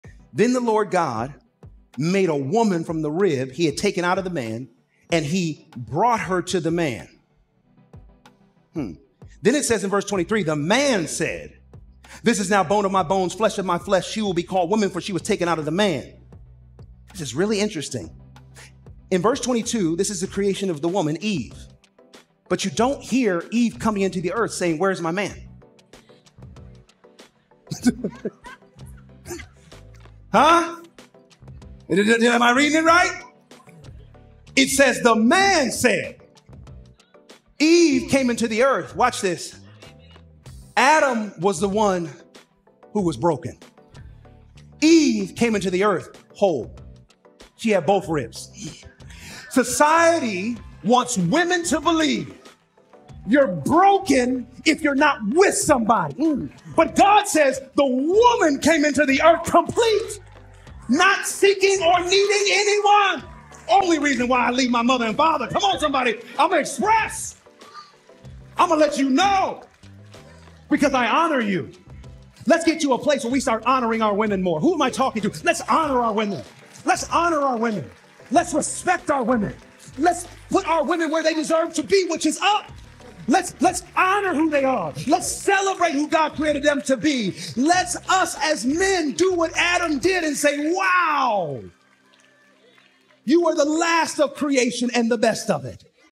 "Eve Did Not Come To The Earth Saying Where Is My Man"
In this sermon by Devon Franklin, he challenges societal beliefs and perceptions about women's completeness. Drawing from the story of Eve's creation, Franklin highlights that although she came from a man, she was inherently whole and complete from the very beginning. He delves into the notion that society often perpetuates the idea that a woman is incomplete without a husband, causing many women to question their worth and value.

Do you believe that a woman is incomplete without a husband?